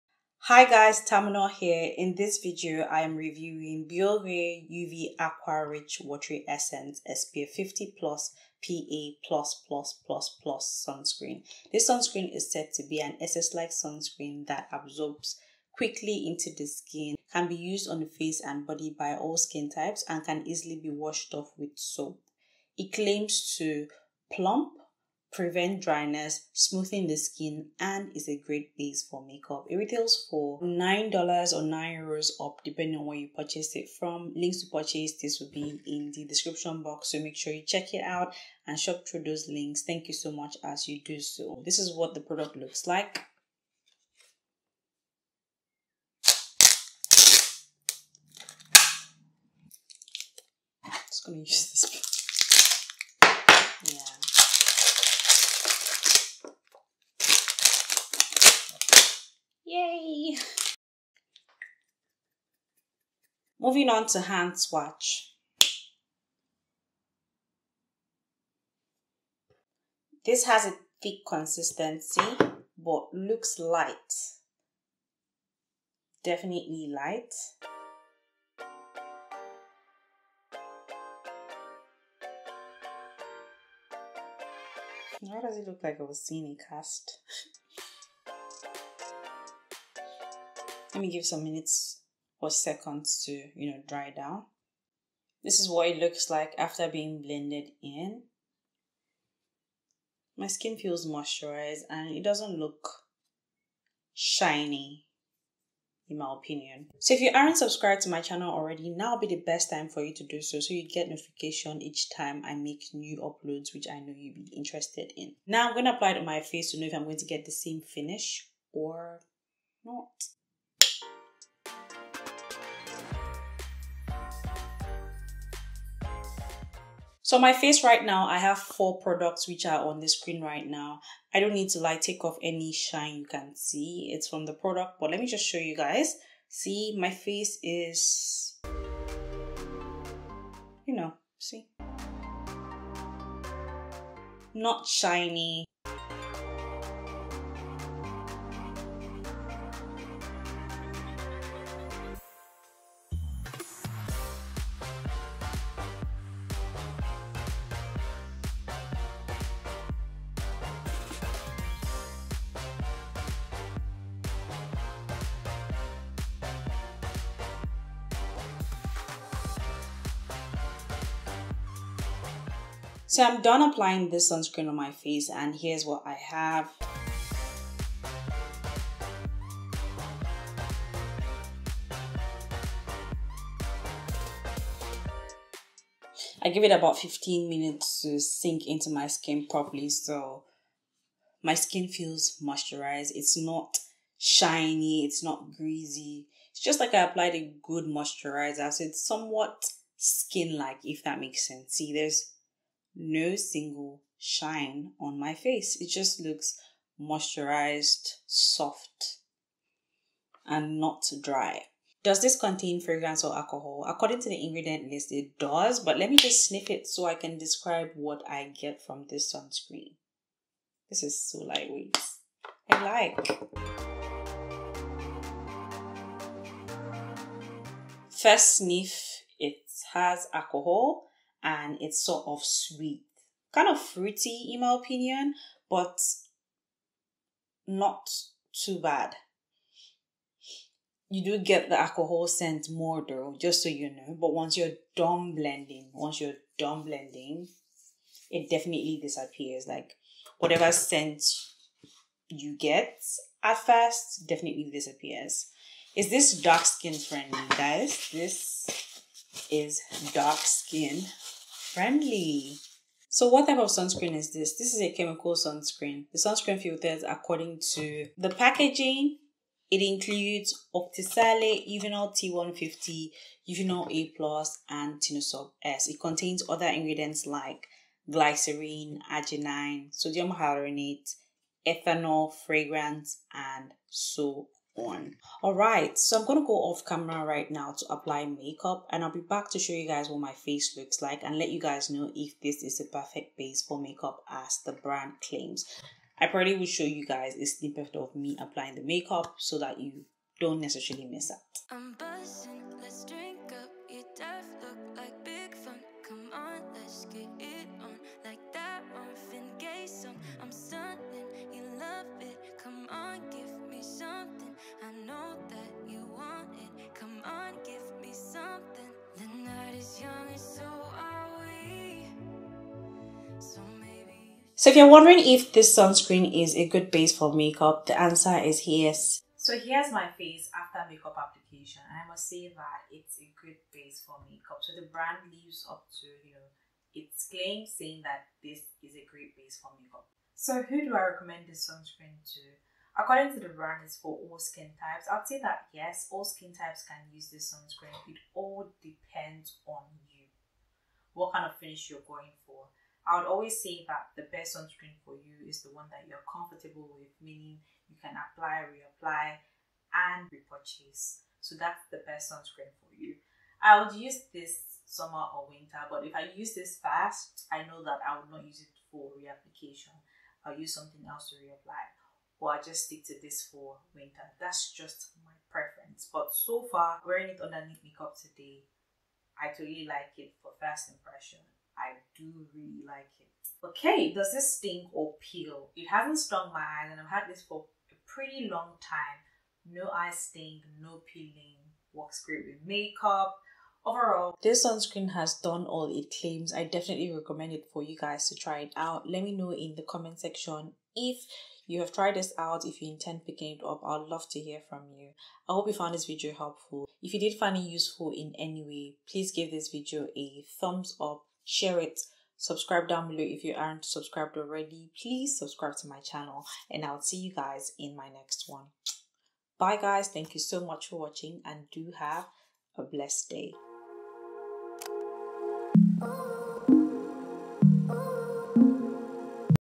Bioré UV Aqua Rich Watery Essence Sunscreen SPF50+ PA ++++ Review
A review of the new Bioré UV Aqua Rich Watery Essence Sunscreen SPF50+ 2023 Edition on dark skin.  This sunscreen is thick yet lightweight, doesn't take time to sink in, and leaves a skin-like finish. It has a sweet alcohol scent, it didn't sting my eyes or pill. Overall, It is a perfect base for makeup.

Laneige Water Bank Blue Hyaluronic Cream Moisturizer - Normal to Dry Skin

Timestamps
00:00 Biore uv aqua rich watery essence sunscreen 2023 reformulation review
01:06 Hand swatch
02:30 Face application
03:11 First impressions
03:32 Fragrance and Alcohol?
04:23 Type of sunscreen
05:01 Is Biore uv aqua rich watery essence sunscreen a good base for makeup?
05:42 Recommendation
06:24 Eye sting or Pilling?

📷 RECORDING TOOLS 🎥
Studio Lights
Tripods
Video Editor: InShot⁣
Thumbnail Design: Canva

“There is only room for growth not perfection”